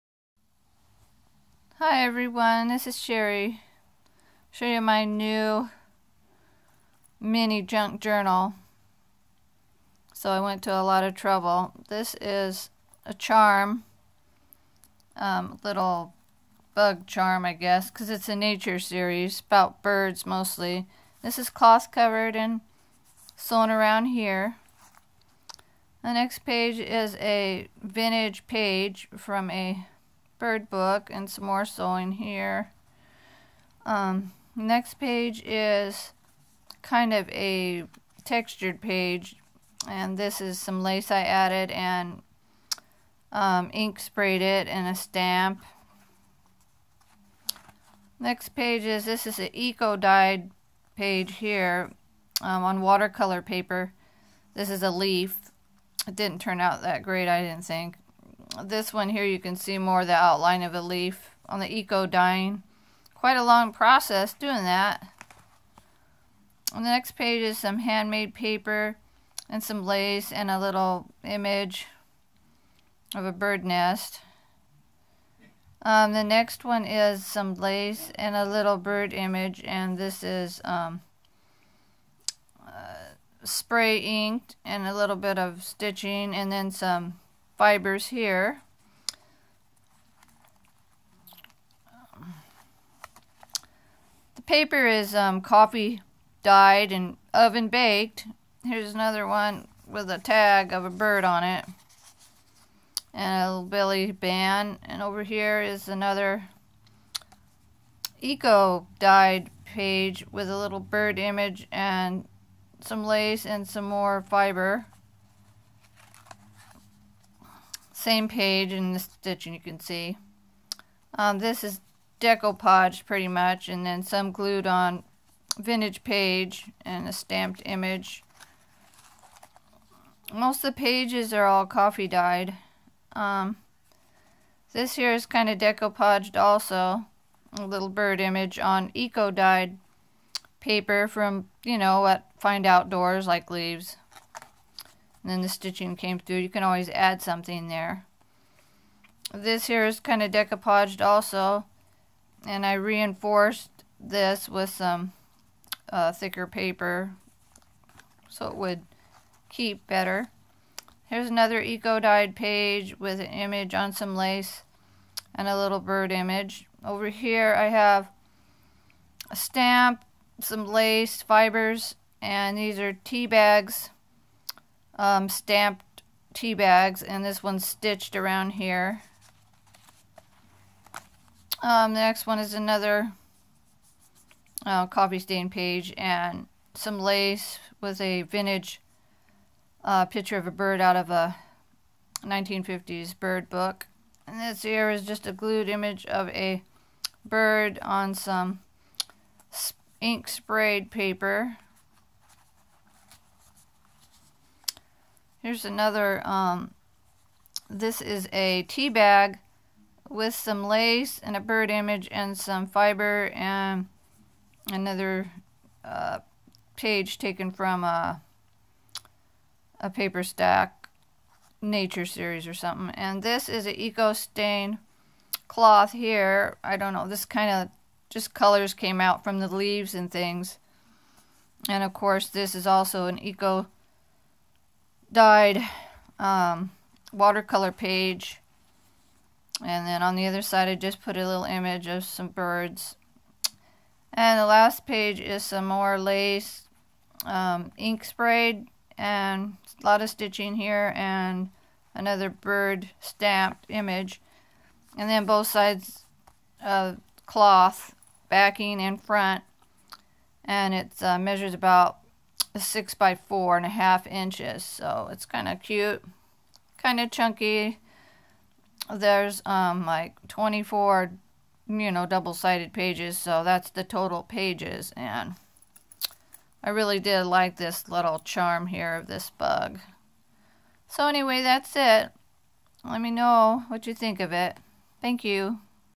Cute Nature bird Junk Journal
Nature bird junk journal contains coffee dyed pages as well as hand dyed eco pages and some fabric dyed, nice collectable in my etsy store